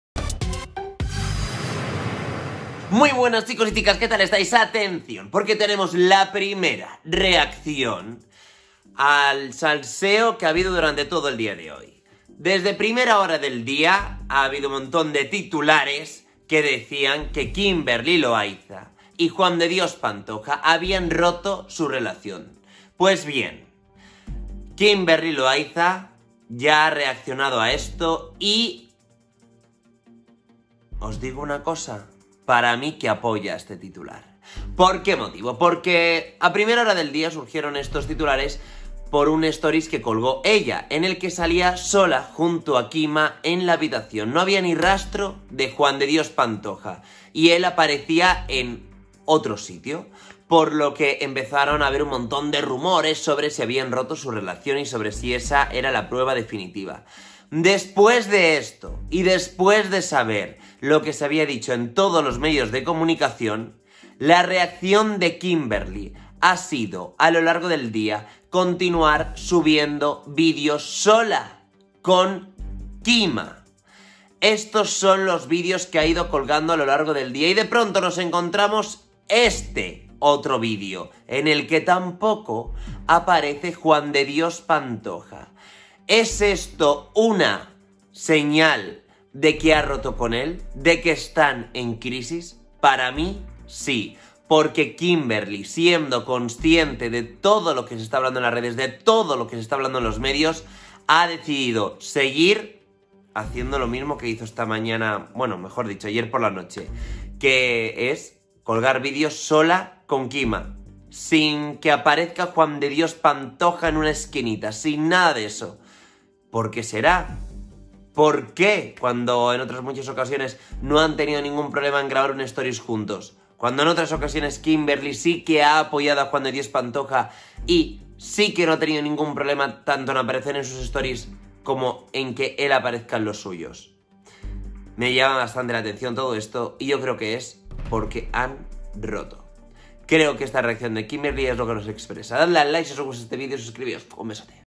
KIMBERLY REACCIONA TRAS SU RUPTURA CON JUAN DE DIOS PANTOJA
¿Es cierta la ruptura de Kimberly Loaiza y Juan de Dios Pantoja tras la supuesta infidelidad que ha sacado a la luz Lizbeth Rodríguez?